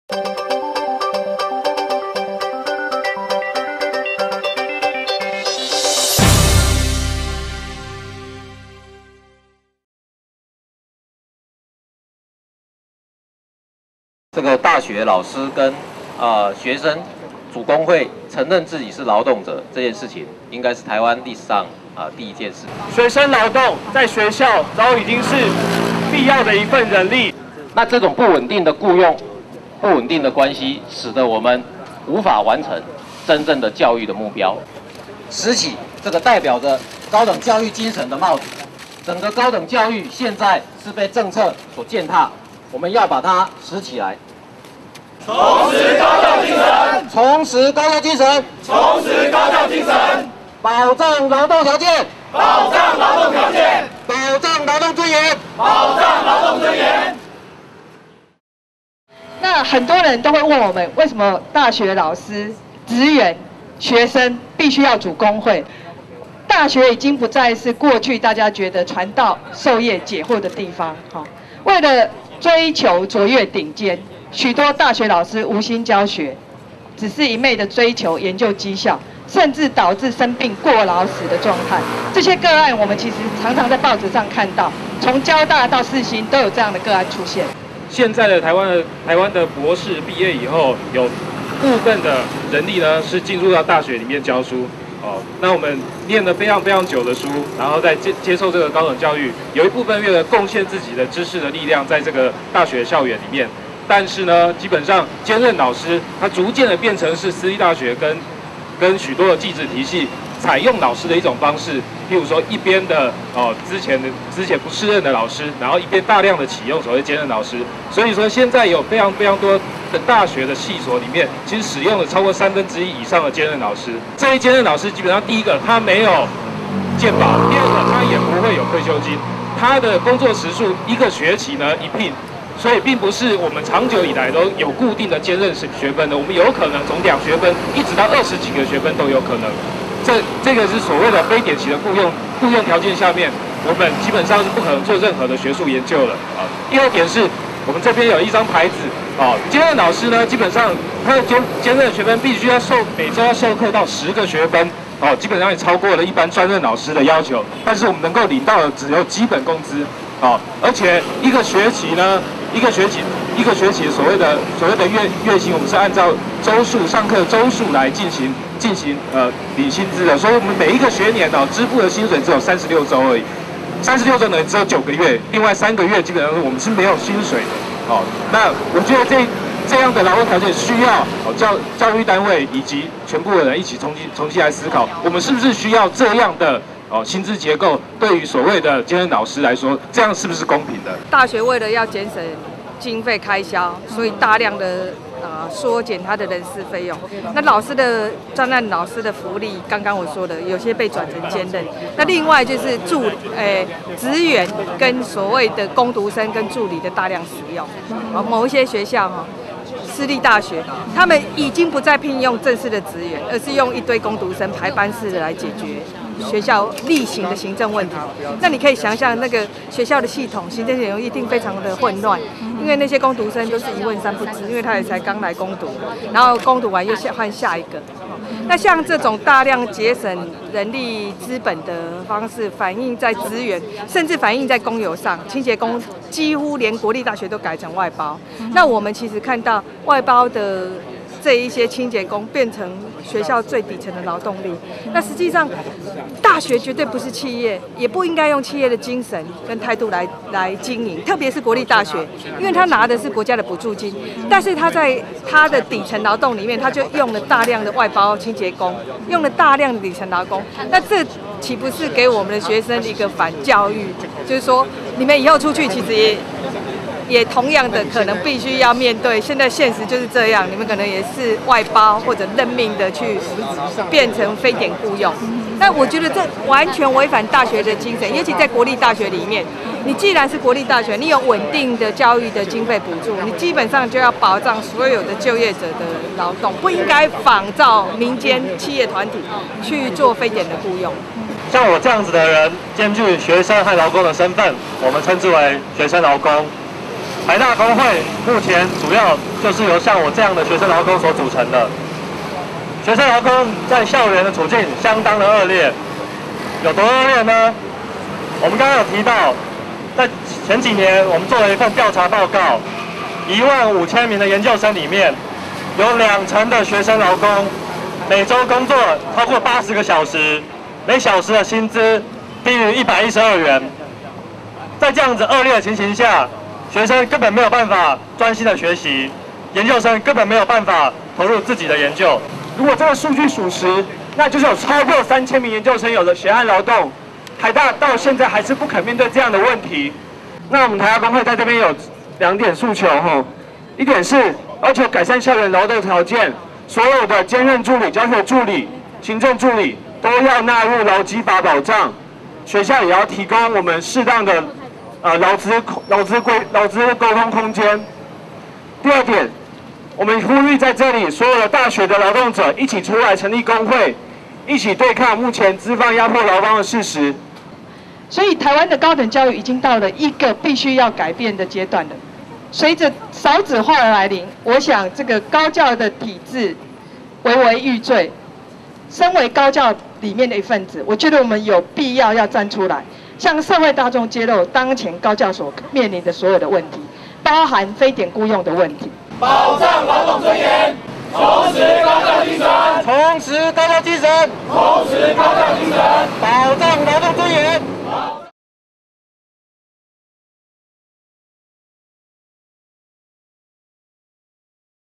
2012.04.30 五一勞動節前集結教育部 高教工會爭校園勞動權益
五一勞動節前，由大學老師與學生組成的「高等教育產業工會」前往教育部，承認校園中勞動者的身分，以高舉學士帽的方式象徵「重拾高等教育精神」，呼籲教育部重視校園勞動者的權益。

輔大社會系教授戴伯芬說，大學為了節省人力資本大量使用外包派遣，行政工作也多用排班工讀生，造成行政工作紊亂；大學中越來越多兼任講師，超時工作又只領有基本薪資。

台大工會理事長黃守達說學生已成血汗勞工，根據他們的調查，有兩成研究生每週超時工作80小時，時薪低於基本工資112元。